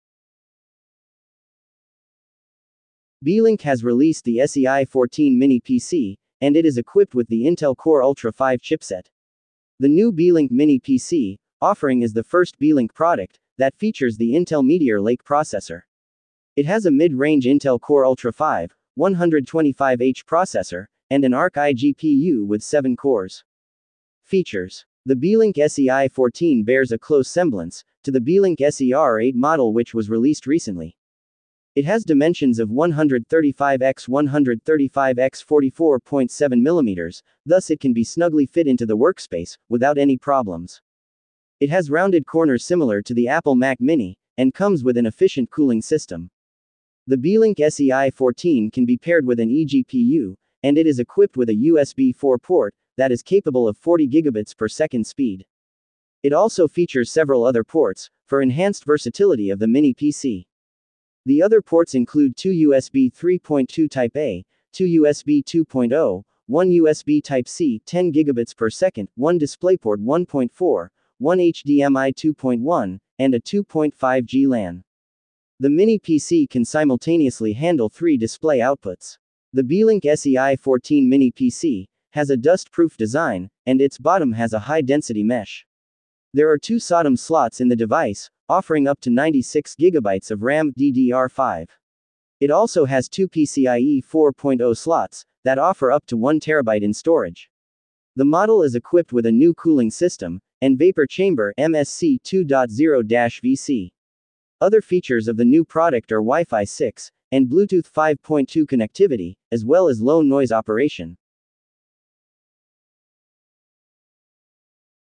Beelink SEi14 mini PC: First Look - Review Full Specifications
Pricing
Beelink is offering the SEi14 mini PC at a starting price of 2,699 yuan ($373) for the barebones model. Global availability and pricing details for the Beelink SEi14 are lacking at this time.